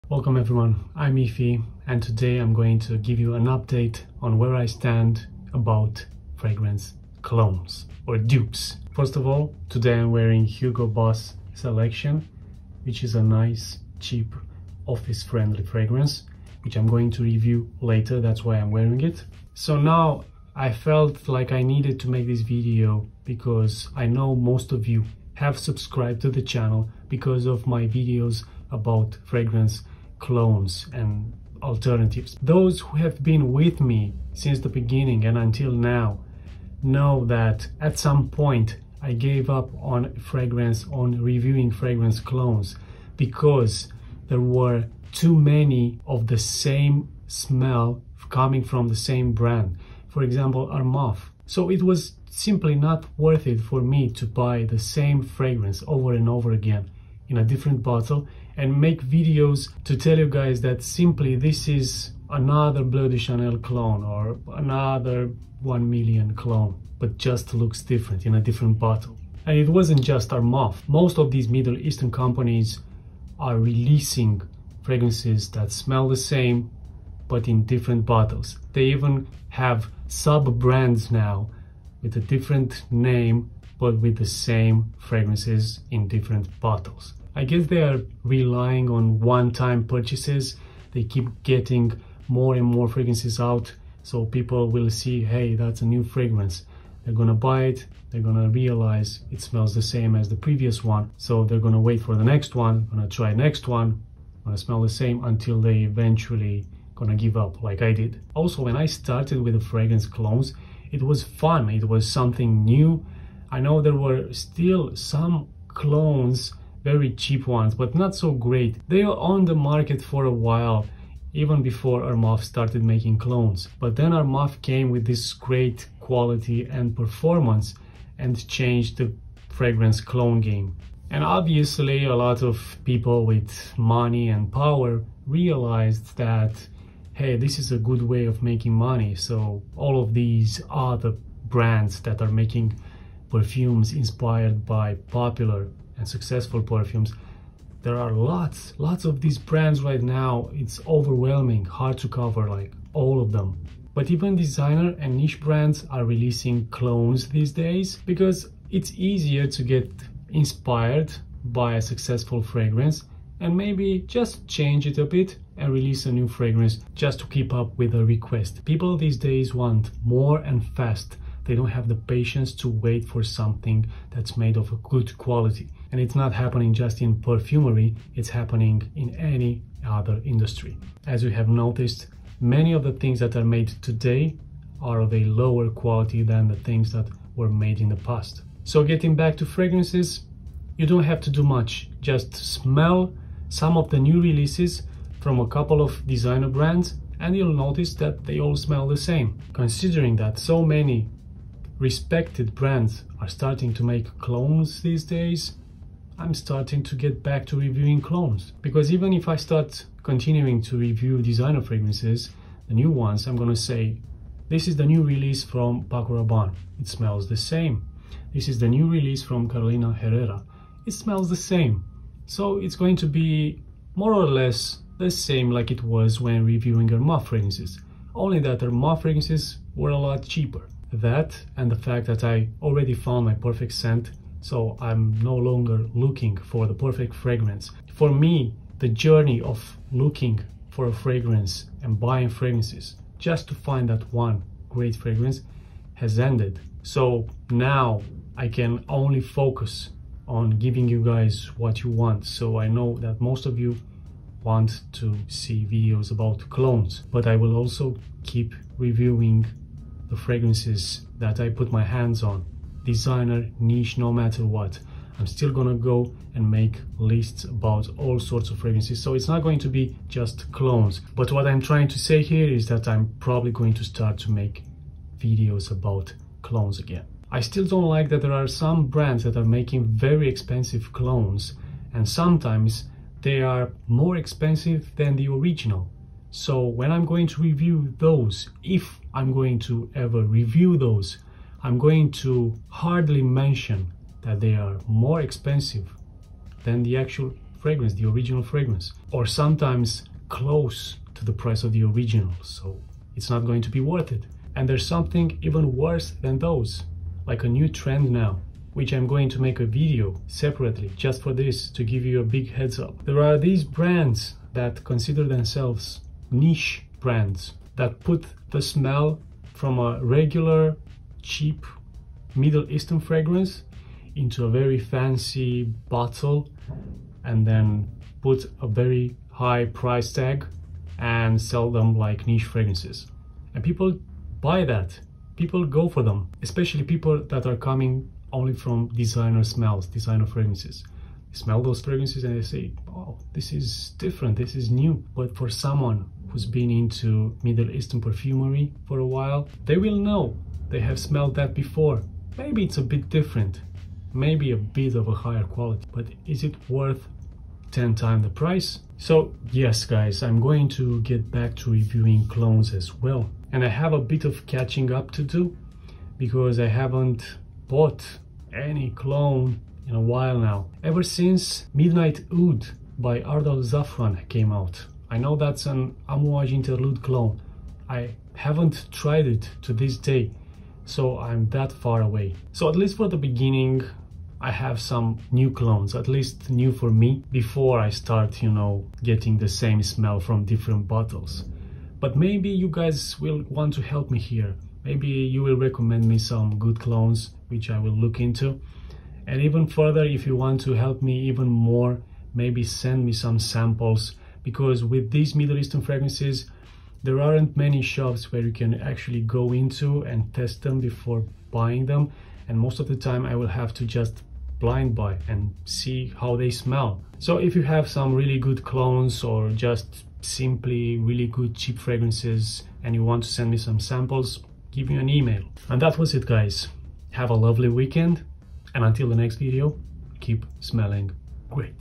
What I Currently Think About Fragrance Clones / Fragrance Dupes
I know many of you have subscribed to my channel because of my videos about fragrance clones or fragrance dupes, so here is an update on where I am now regarding perfume clones.

My current favorite fragrances:
1. Clive Christian 1872 for men
4. Acqua di Parma Essenza
5. The Neroli Portofino DNA:
7. Guerlain L'Homme Ideal Cologne
9. Versace Man Eau Fraiche
My equipment:
Camera: iPhone 12 mini
Microphone: Rode Videomic Pro
Mount:
Ulanzi tripod
Ulanzi cold shoe mount
Lights: Ulanzi RGB